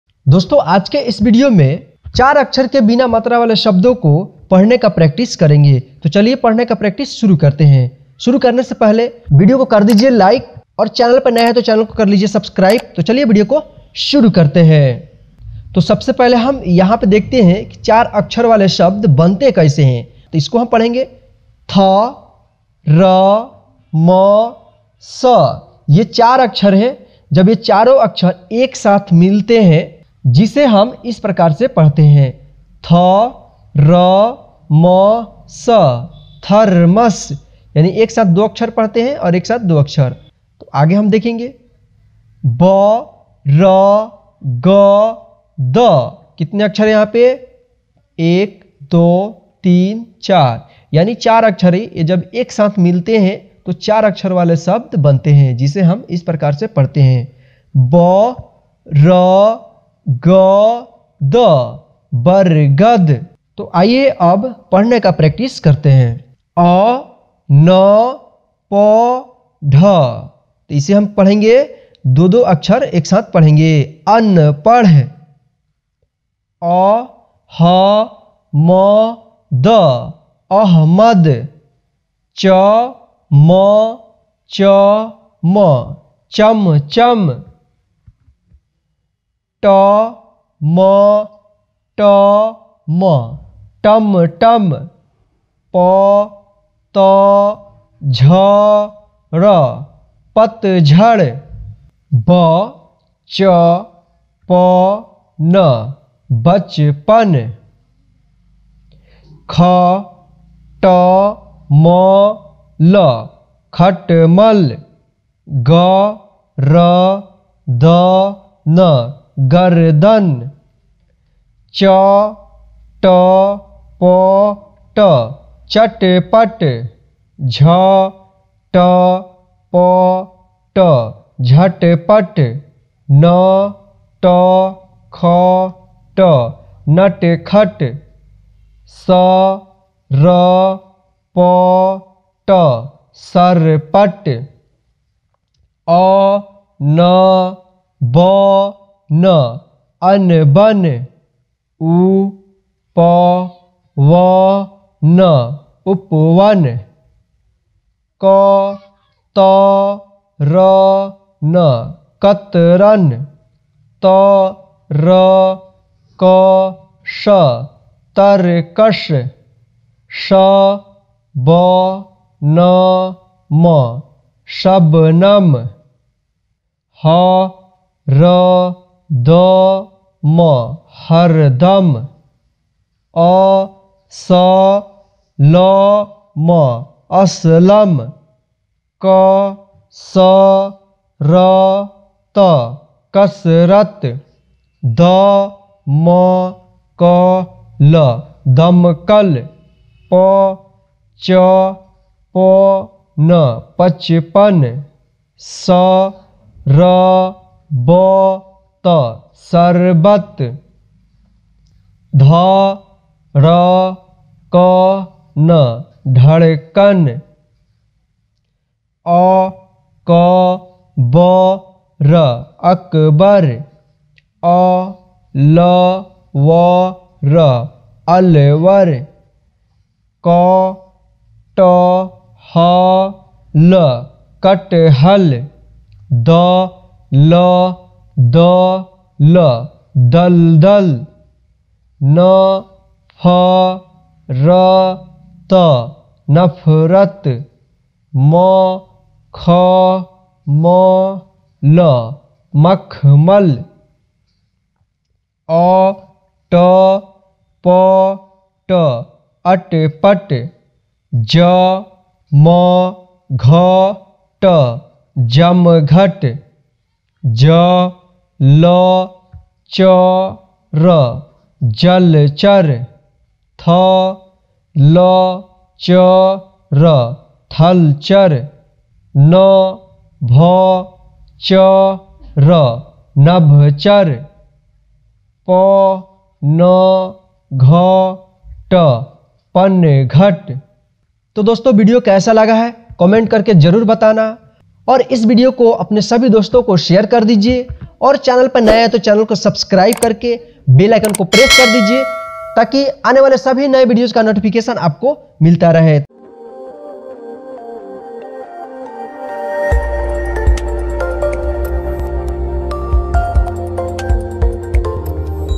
Hindi Reading Practice l हिंदी पढ़ना सीखे l Recognize The Words in Hindi l चार अक्षर वाले शब्द
Hindi Reading Practice l हिंदी पढ़ना सीखे l Recognize The Words in Hindi l तीन अक्षर वाले शब्द

I'm Md Shakil welcome to on my YouTube channel "Genius Shiksha"

dosto maine is video me Char akshar wale shabd ko padhne ka practice karaya hu taki aap hindi padhna sikh jaye.